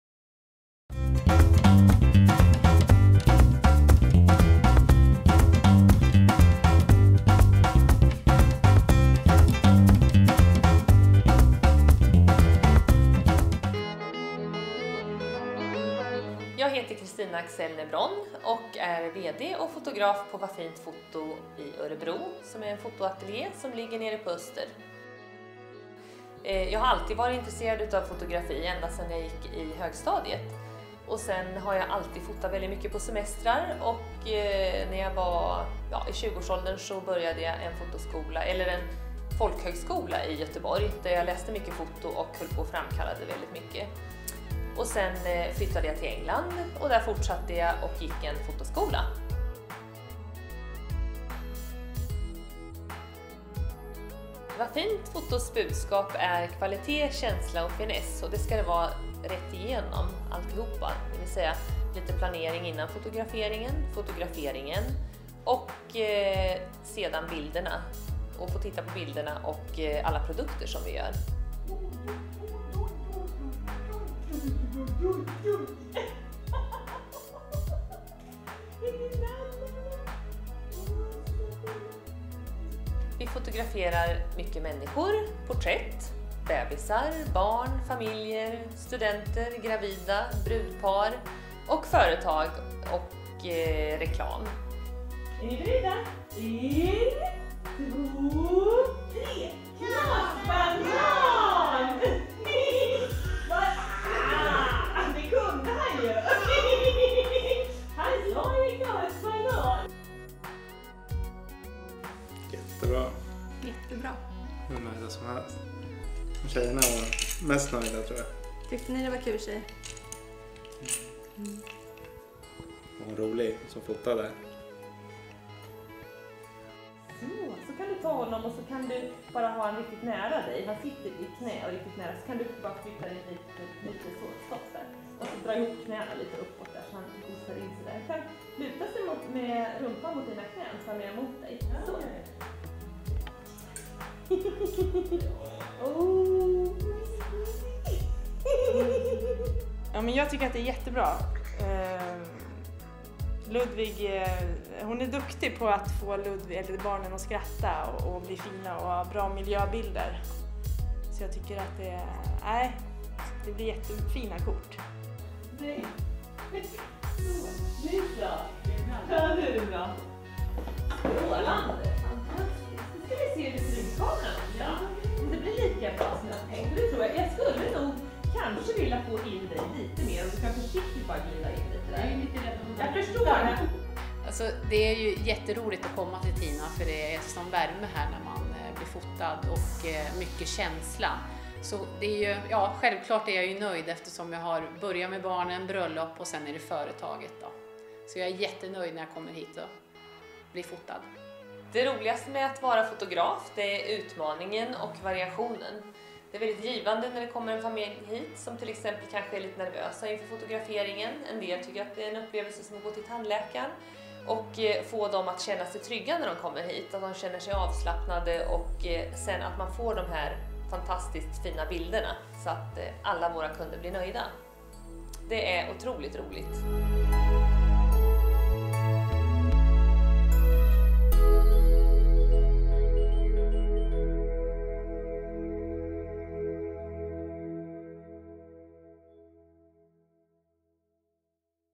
Va Fint Foto - Informationsfilm
Digital Mediedesign III - Projektarbete; bakomfilm på Örebro Universitet för Va Fint Foto. Av Beatrice Lind och Zahraa Jalil VT 2012.